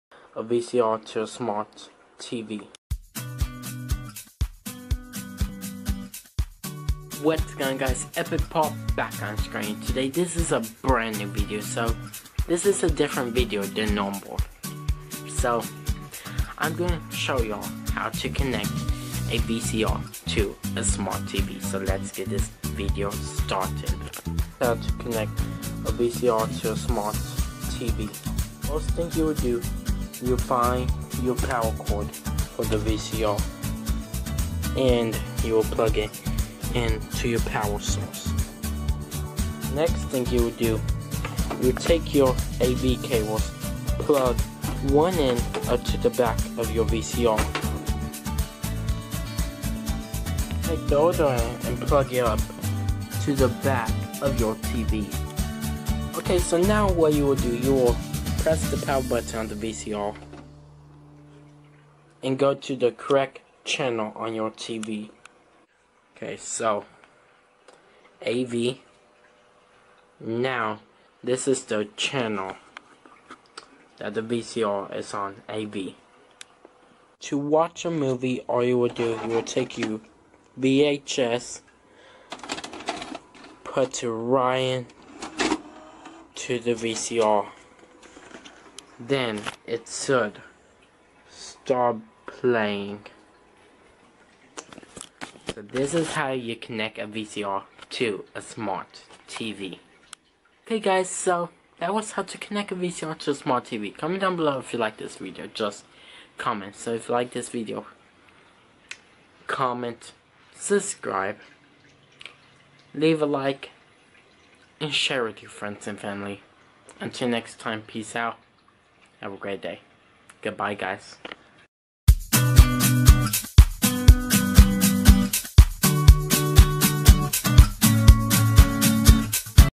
How To Connect VCR To Smart Tv...
Ok guys so this is a video of how to Connect a VCR To a Smart tv so if you like this video Comment Down below subscribe if you new Until next time peace out and have a good Day!!!